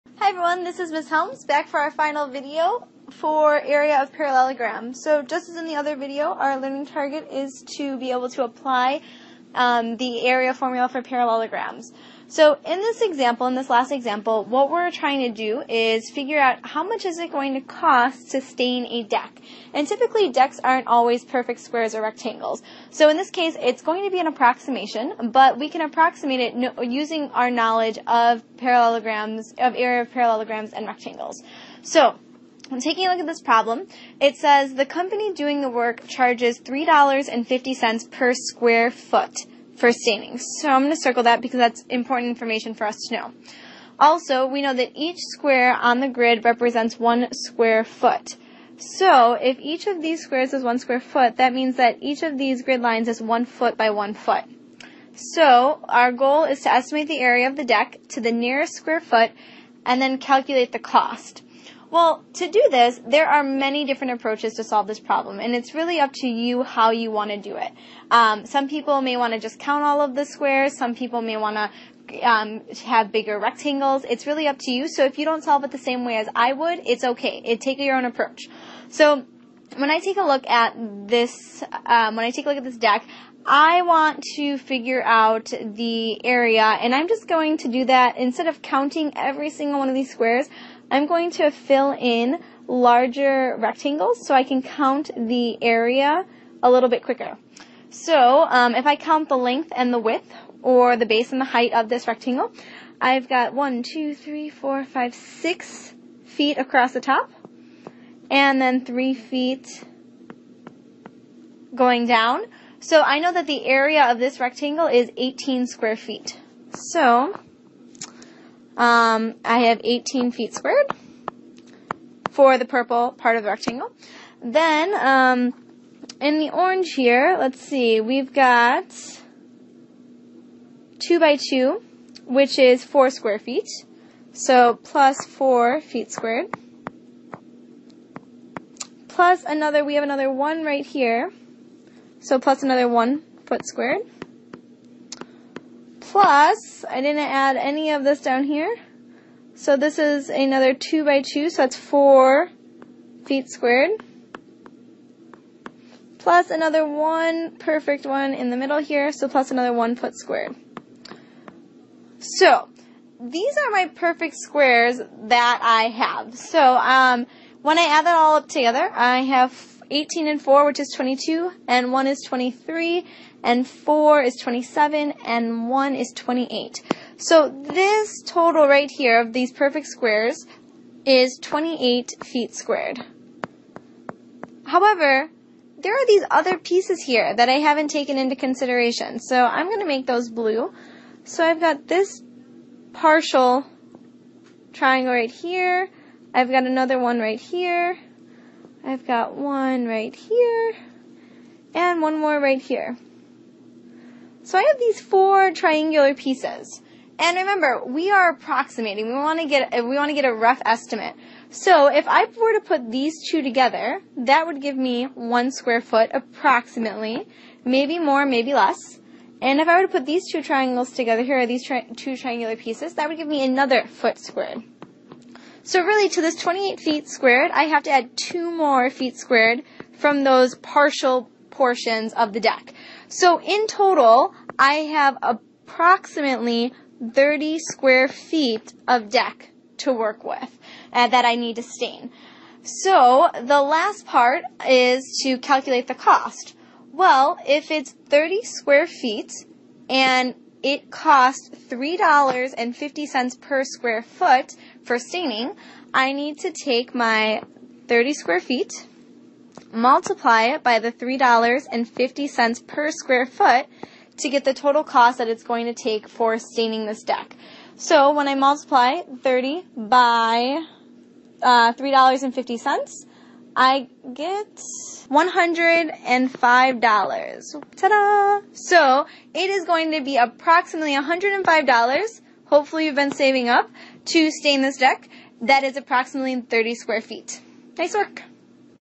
Area of Parallelograms Video #3
Solve application problems involving area of parallelograms.